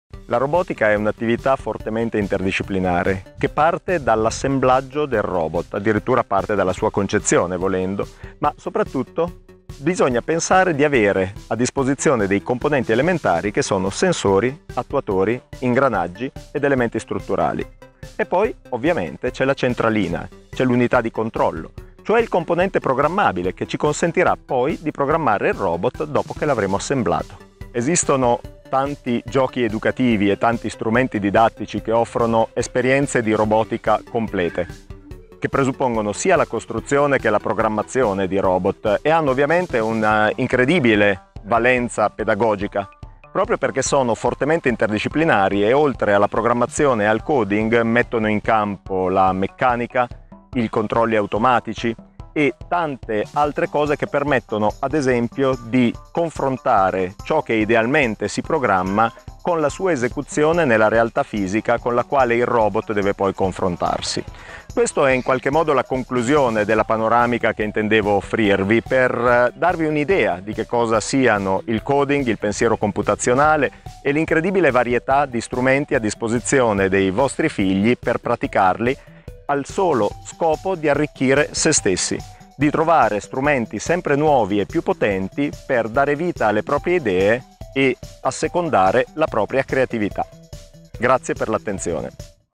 Coding per genitori - 10. Robotica educativa
CodyParentsMOOC 10.
Brevissimo MOOC per genitori tenuto da Alessandro Bogliolo per l'Università di Urbino.